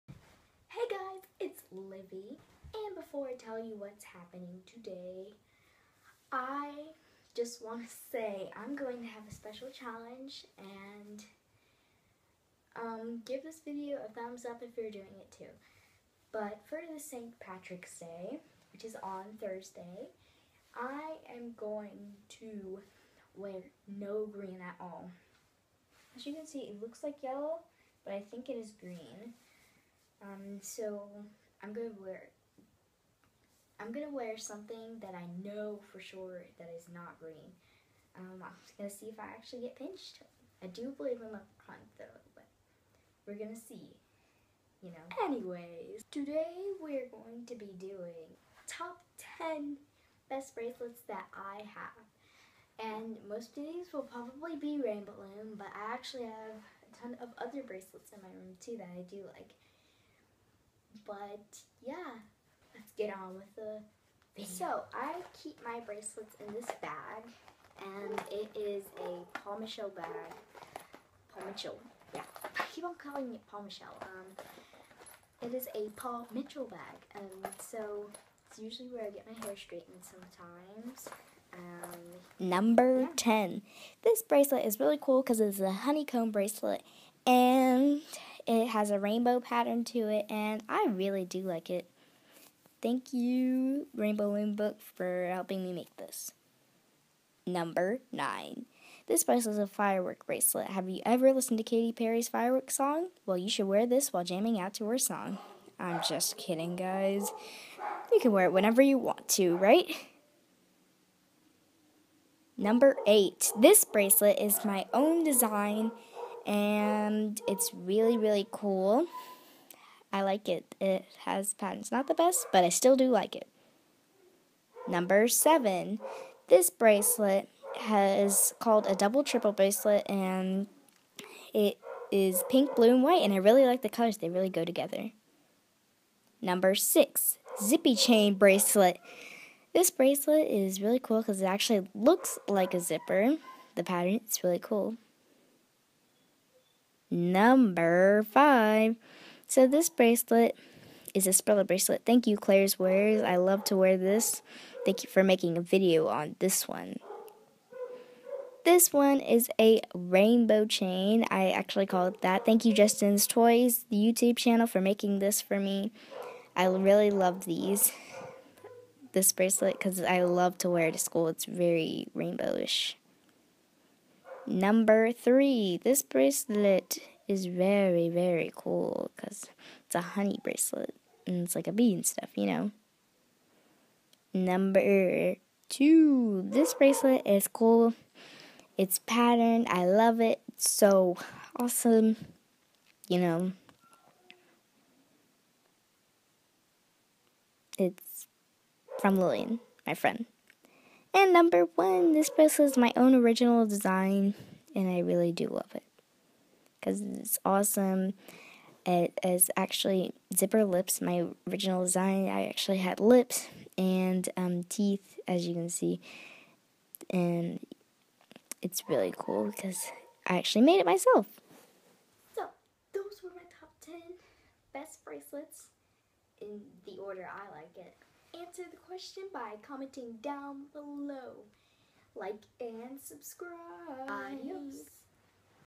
My Top Ten Most Special Bracelets in my Room
It's Livi! This is a list of the top ten best bracelets I have. #2 is actually one of my friends bracelets that she left at my home. She never really visits anymore. Oh well!

The shoutout question is:
What is your most special bracelet?
Answer this and you could be in the Shoutouts next video!